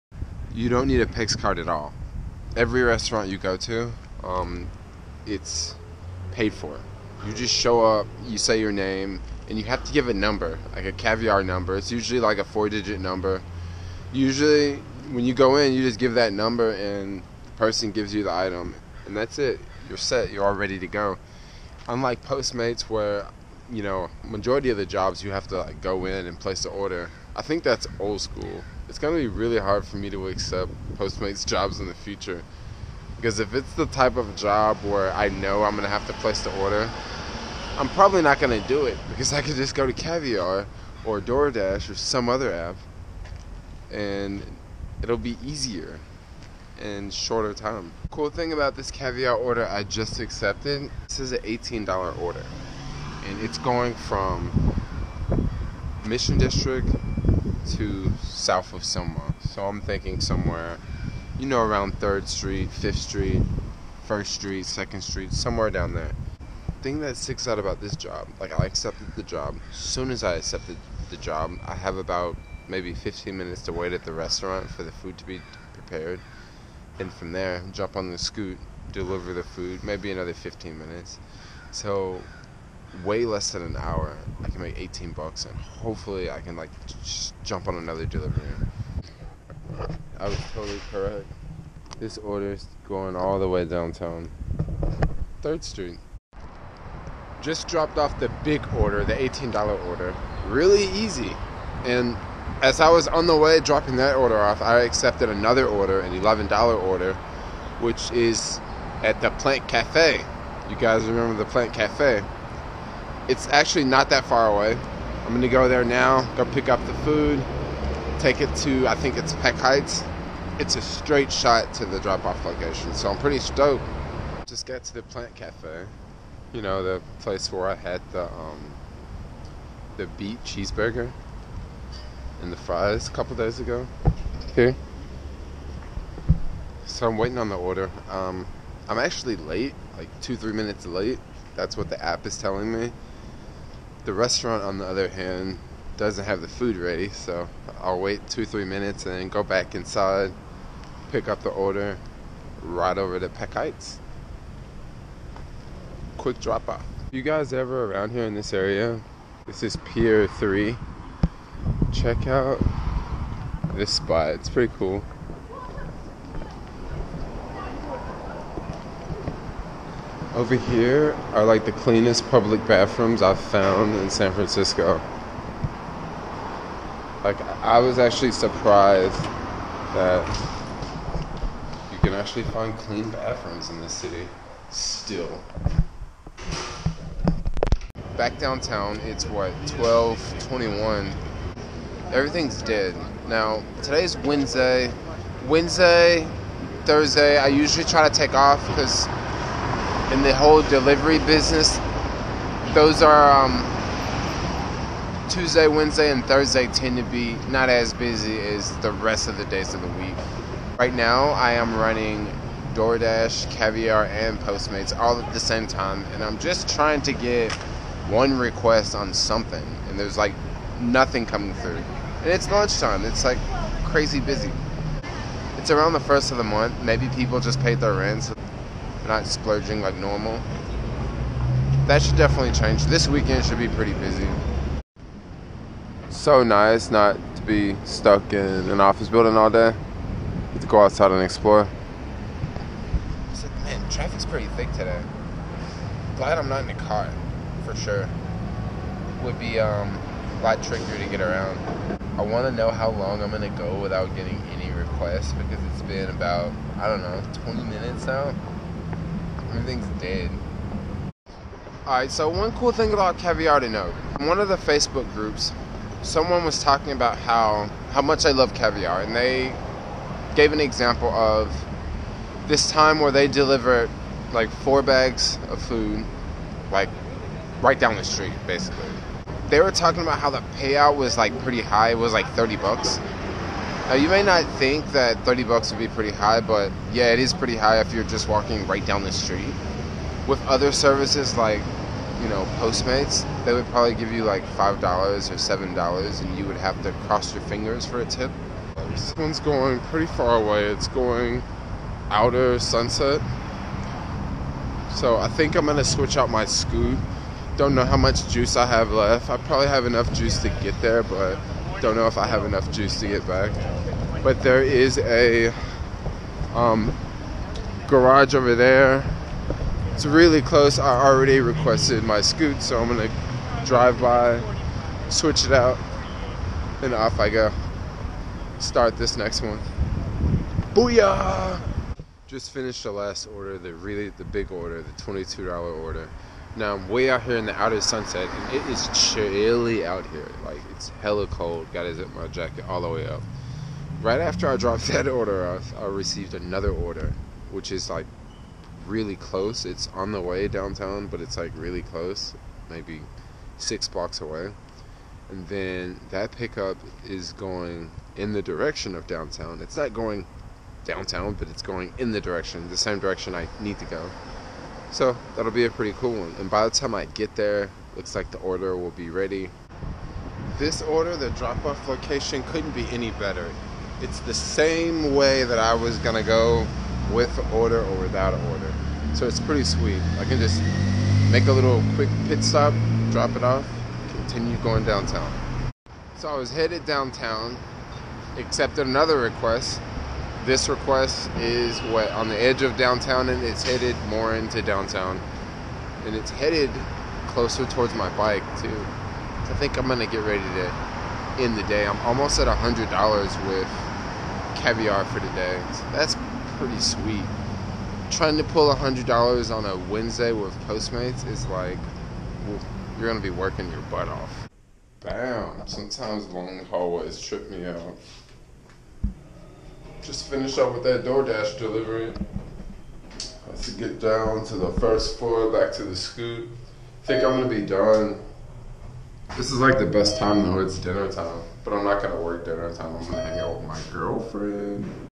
Caviar Food delivery first day VLOG 60
Another bay area start up Caviar very similar to Postmates. Not it all.

Filmed in San Francisco
Follow me everywhere!